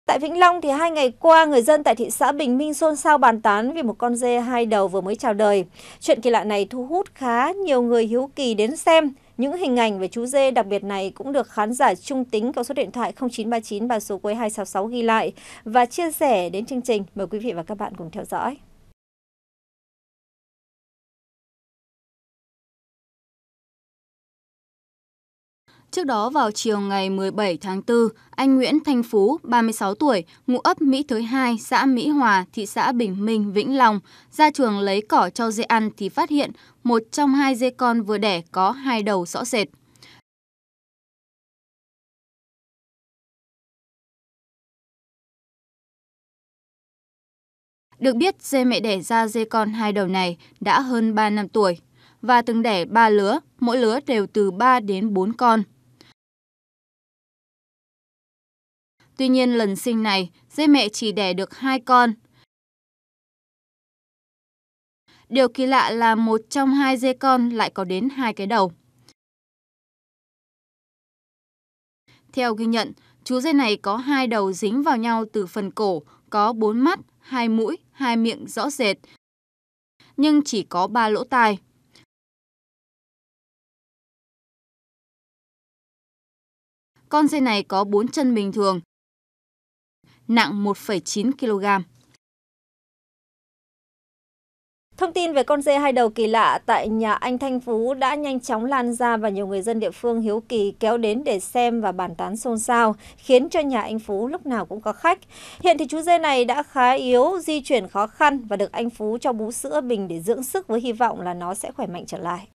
Kỳ lạ chú dê 2 đầu tại Vĩnh Long| VTC14
VTC14 | tại Vĩnh Long, hai ngày qua, người dân Thị xã Bình Minh xôn xao bàn tán về một con dê 2 đầu vừa chào đời. Chuyện kỳ lạ này thu hút nhiều người hiếu kỳ kéo đến xem.